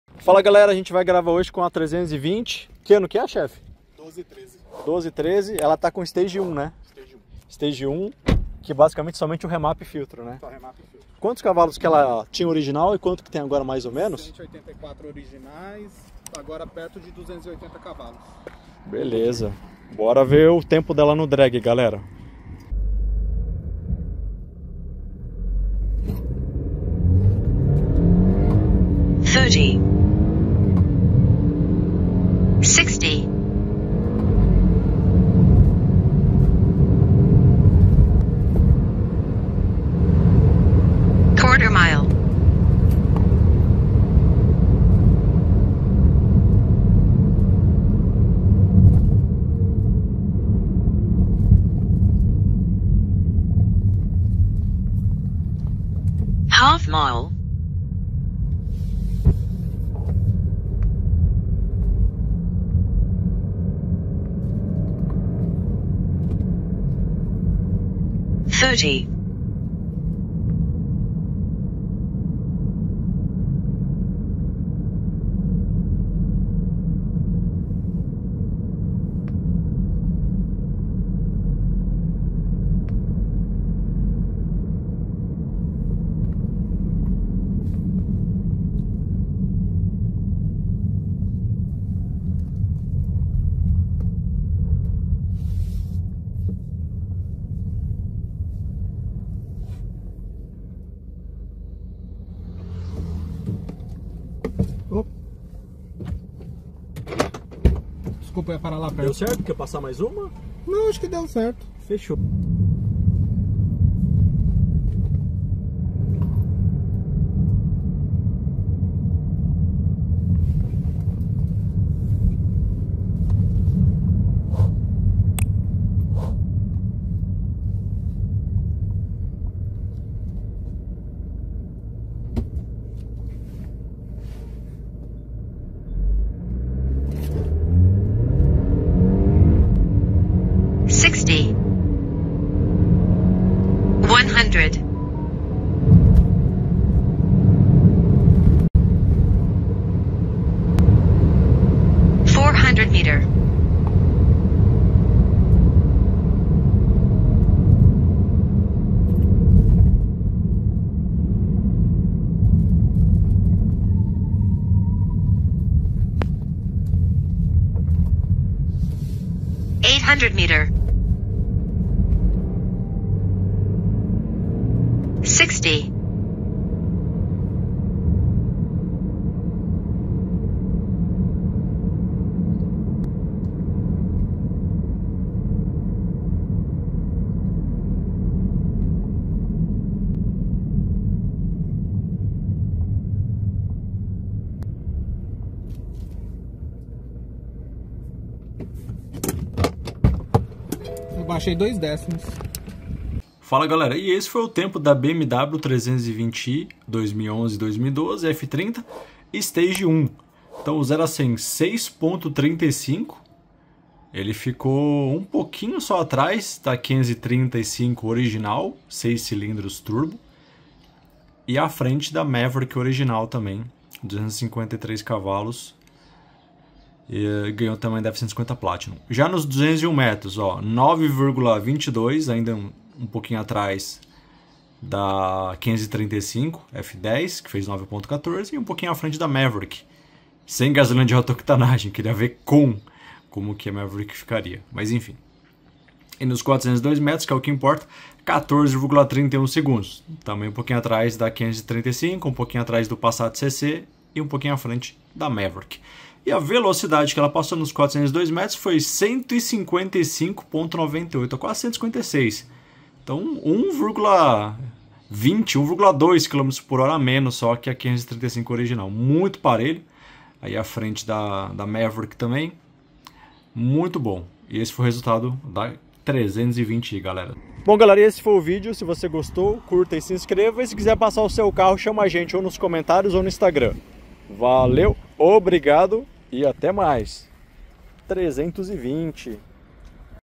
BMW 320i 184cv acelerou tudo 0-100, 201m e 402m
F30 com remap pisando tudo no Dragy. Ranking completo